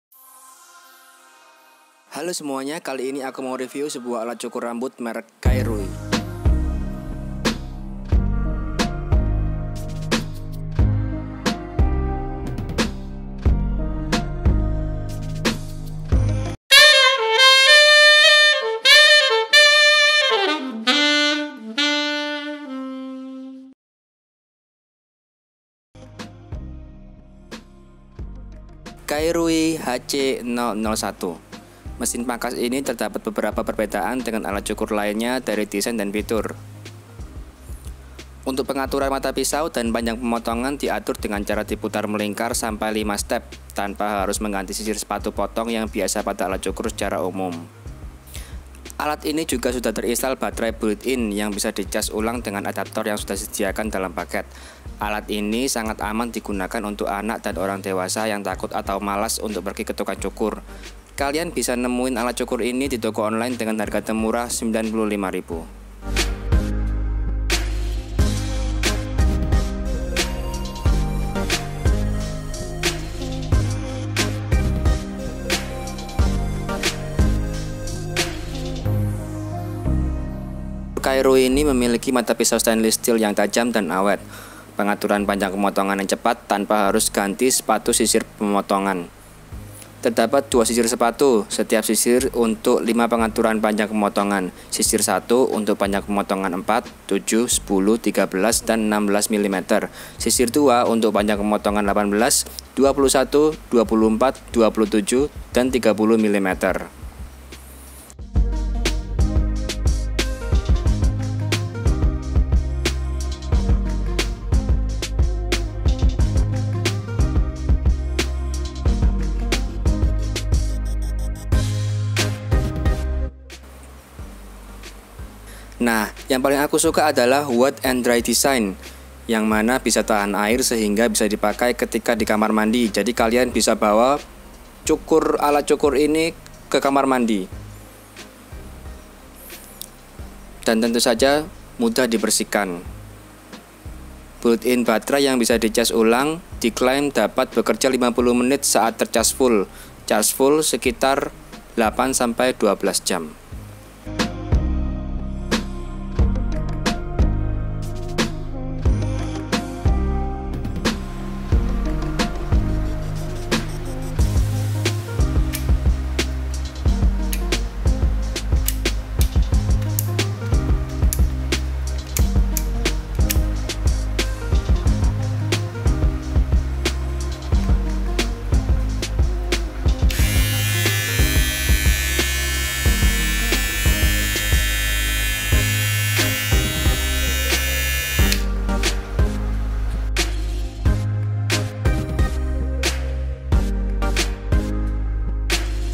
Hair Clipper / Pencukur Rambut Kairui HC-001 | REVIEW
Hair Clipper / Pencukur Rambut Kairui HC-001

Mesin pangkas ini terdapat beberapa perbedaan dengan alat cukur lainnya dari desain dan fitur.
Untuk pengaturan mata pisau dan panjang pemotongan diatur dengan cara diputar melingkar samai 5 step tanpa harus mengganti sisir sepatu potong yang biasa pada alat cukur secara keumumam dan alat ini juga  Untuk sumber tegangan sudah terinstal baterai (built in) yang bisa dicharge ulang dengan Adaptor yang sudah disediakan dalam paket. Alat ini sangat aman digunakan untuk Anak dan orang dewasa yang takut atau malas untuk pergi ke tukang cukur. kalian bisa nemuin alat cukur ini di toko online dengan harga termurah 95rb.